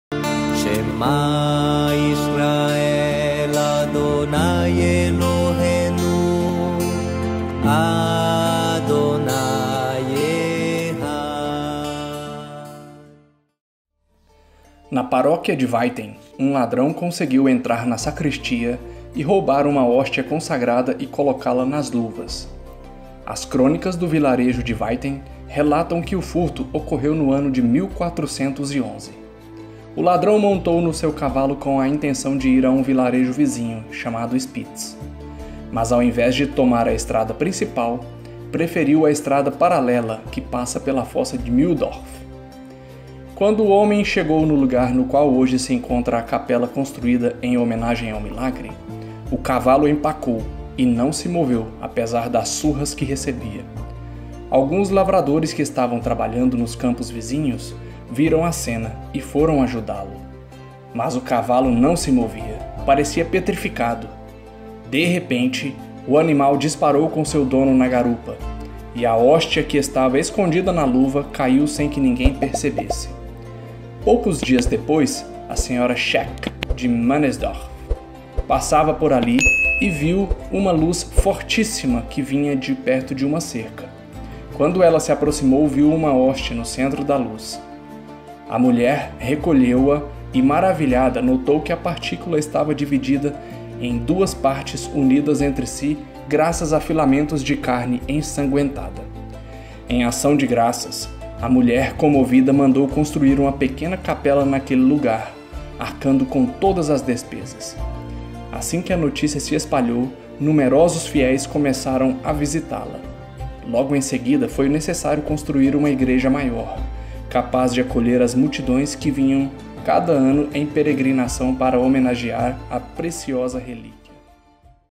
Milagre Eucarístico Áustria 1411 (Eucharistic Miracle Austria 1411)
No Século XV ocorreram na Áustria muitos furtos de Hóstias Consagradas e por esta razão os religiosos começaram a guardá-las nas sacristias. Apesar dessa precaução, no ano de 1411 um ladrão conseguiu roubar uma Hóstia Consagrada da paróquia de Weiten. A Partícula caiu no chão sem que o ladrão percebesse e dias depois foi achada por uma piedosa senhora.
A Hóstia era radiante, estava dividida em duas partes mas unidas graças a filamentos de carne  ensangüentada (In the 15th century, many thefts of Consecrated Hosts took place in Austria and for this reason the religious began to keep them in the sacristies. Despite this precaution, in the year 1411 a thief managed to steal a Consecrated Host from the parish of Weiten. The Particle fell to the ground without the thief noticing and days later was found by a pious lady.
The Host was radiant, it was divided into two parts but joined thanks to threads of bloody flesh.).